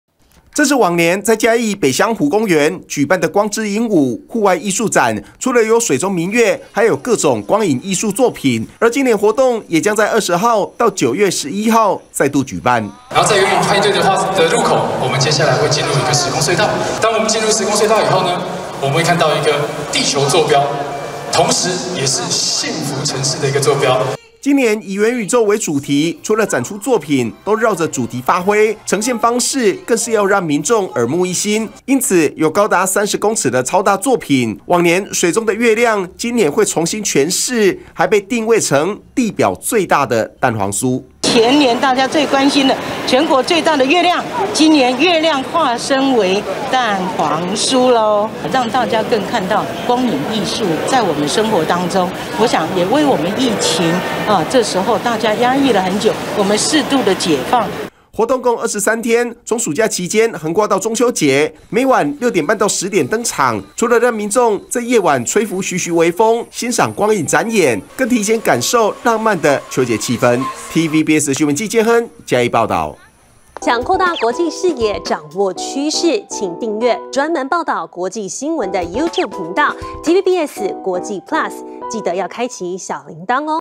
賞地表最大蛋黃酥 嘉義市限定「光織影舞」藝展8/20夢幻登場｜TVBS新聞
嘉義市「光織影舞」戶外藝術展，8月20號'盛大登場，今年以「元宇宙」為主題，水中的月亮，變身地表最大蛋黃酥，還有全台最大的LED立體矩陣及迴路，並以3萬6千顆LED燈，打造巨型創作。